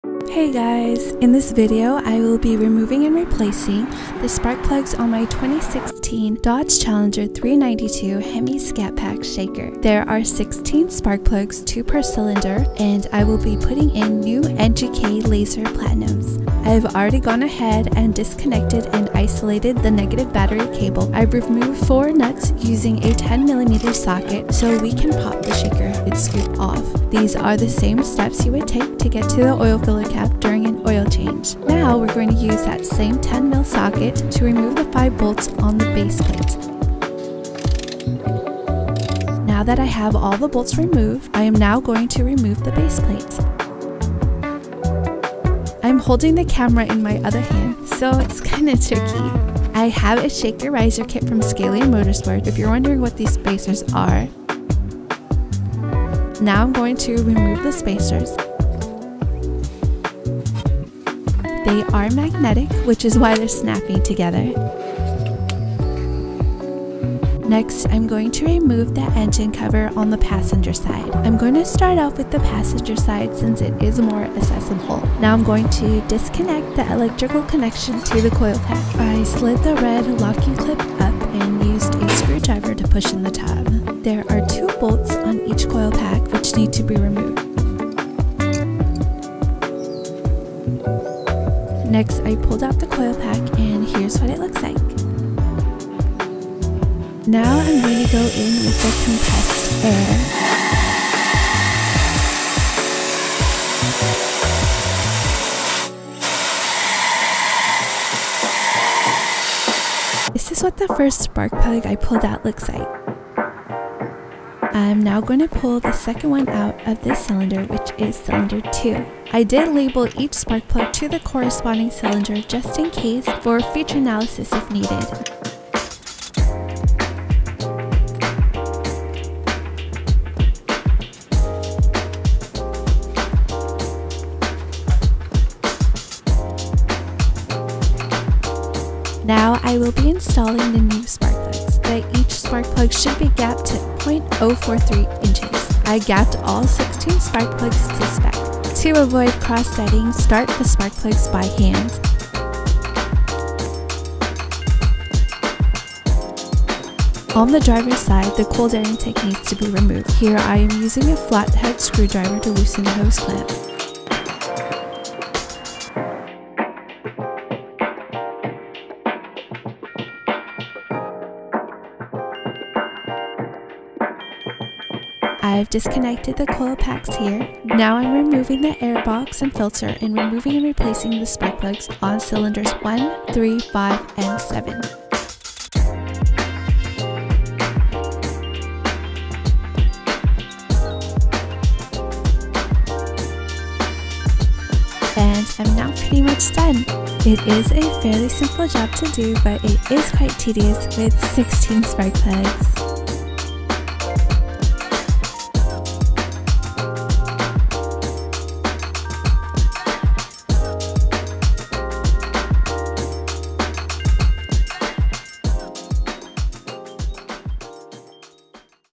DODGE CHALLENGER 392 HEMI SCAT PACK SHAKER SPARK PLUG REPLACEMENT
In this process video I am removing and replacing the spark plugs on my Dodge Challenger 392 Hemi Scat Pack Shaker. I didn't show every single step but you get the idea!